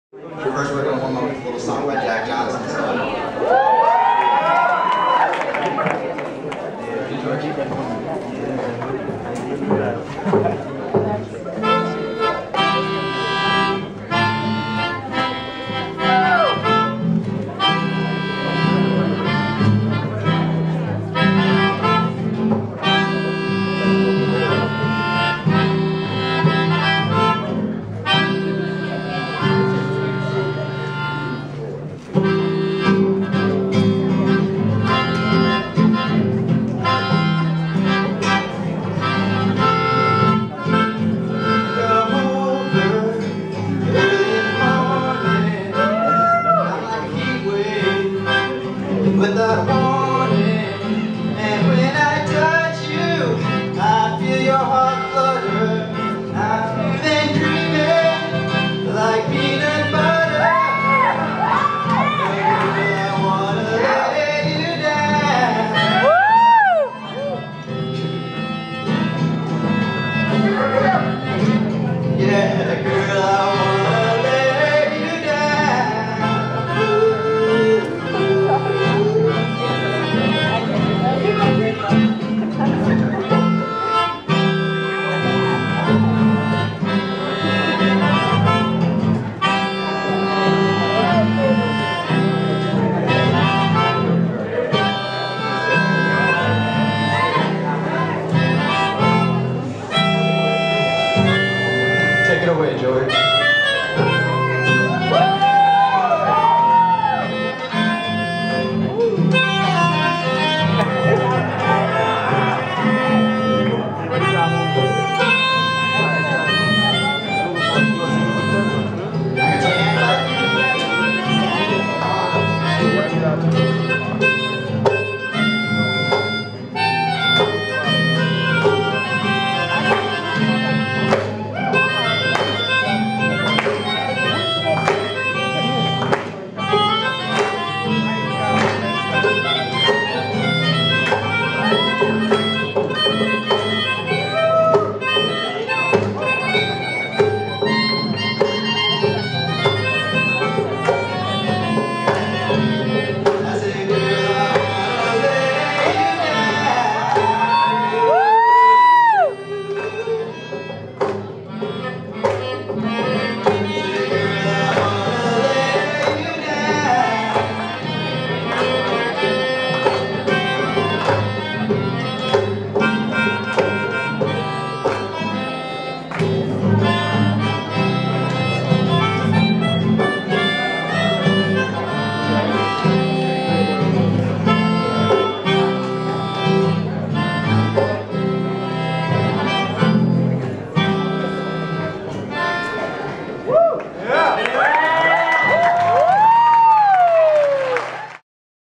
Tim and George - guitar melodica (jack johnson cover)
jack johnson cover

tim edmiston - guitar |  george marzloff - melodica